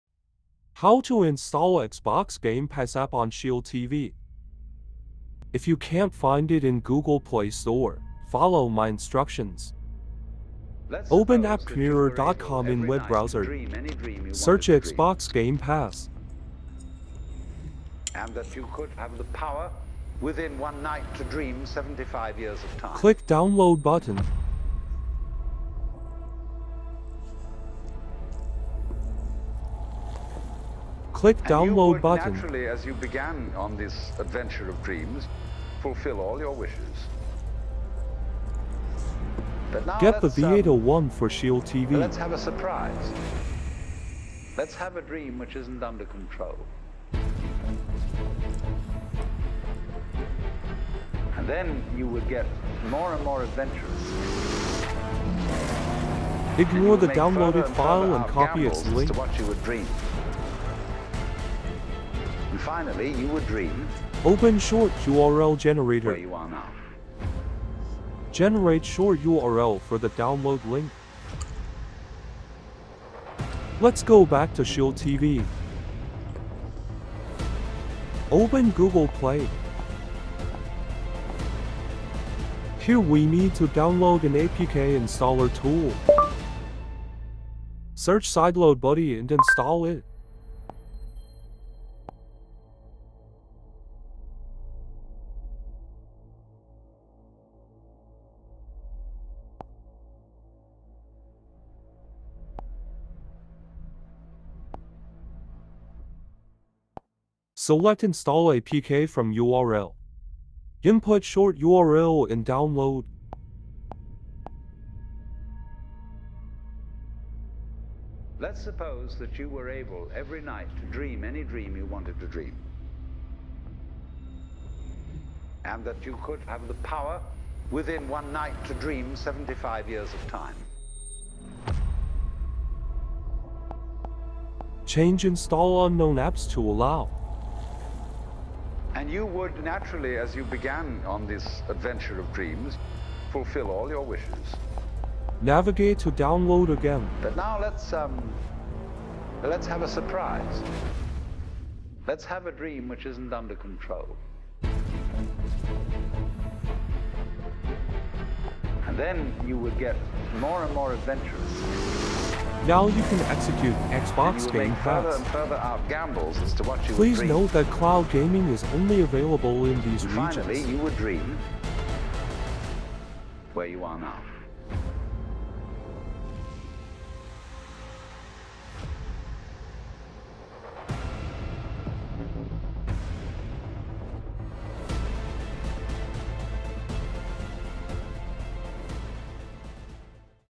How to install MICROSOFT XBOX GAME PASS on NVIDIA SHIELD TV
How to sideload XBOX GAME PASS on NVIDIA SHIELD TV android tv 11 device if you can't find it in Google Play store for TV? Follow the instructions to sideload it, please note that you still need to check if your region is supported for cloud gaming.